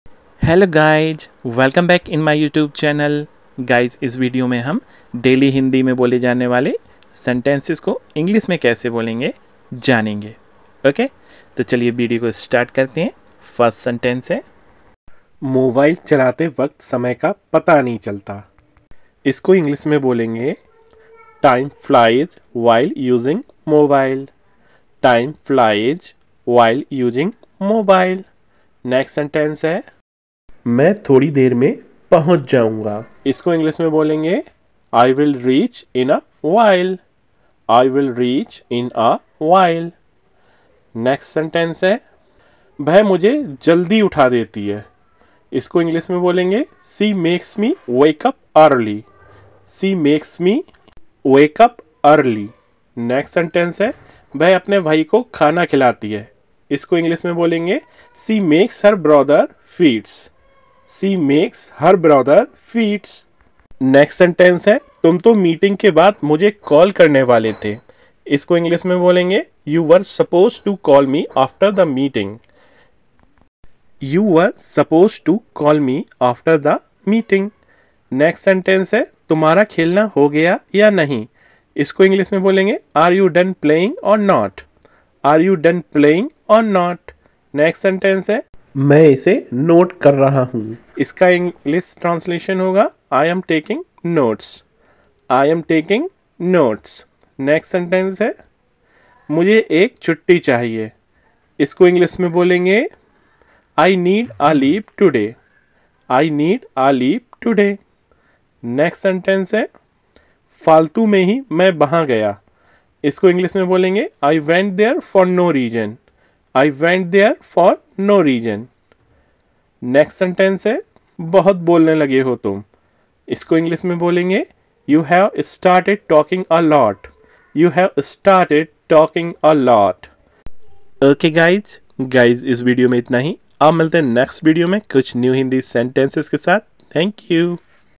English speaking practice | Daily use sentences | English speaking course | Part -8
In this video , you will know english translation of Hindi sentences .